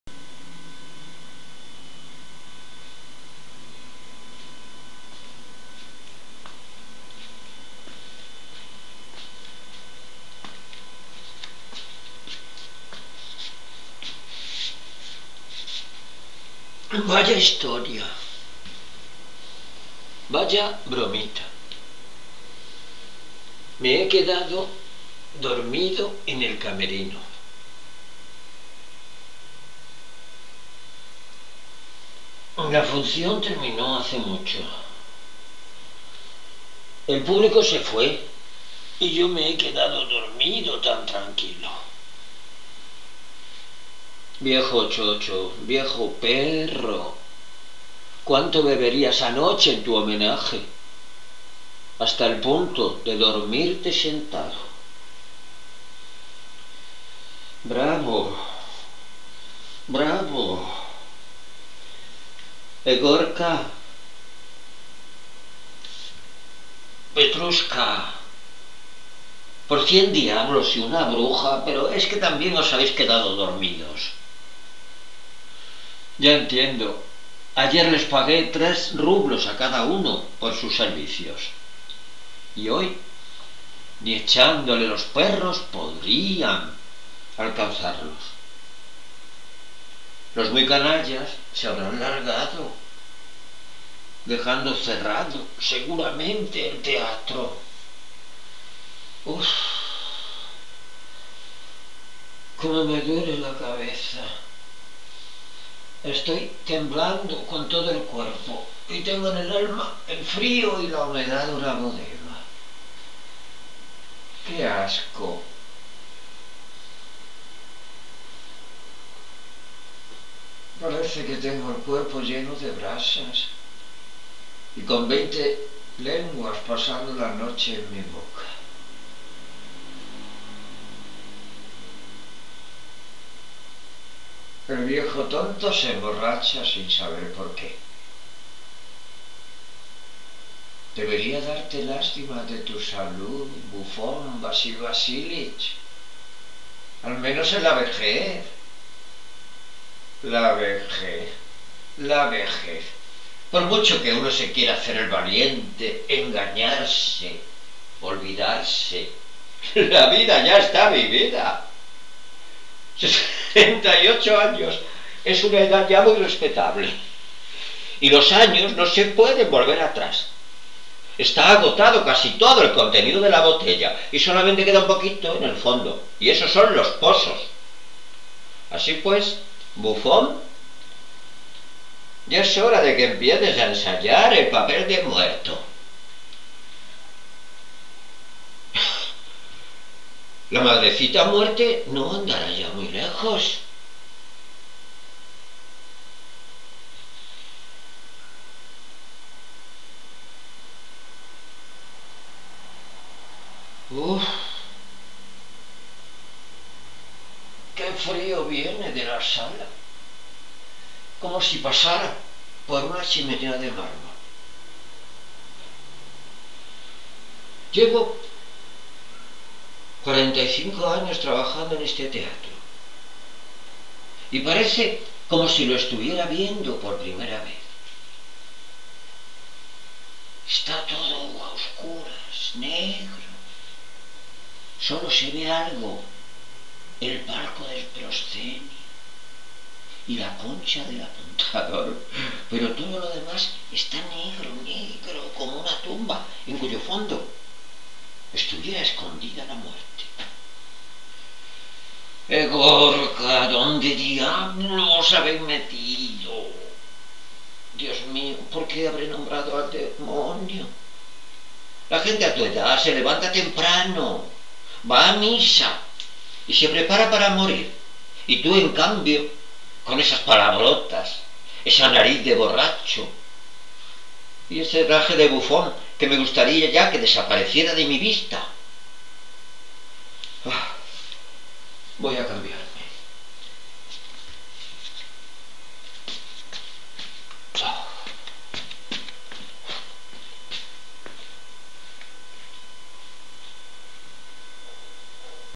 EXPLORANDO EL COMIENZO DEL CANTO DEL CISNE DE ANTÓN CHEJOV.
"EL TEATRO COMO CUALQUIER ARTE NECESITA RETOS, ES DECIR SEGUIR BUSCANDO PUES NO ESTÁ TODO DICHO."Rumboso2.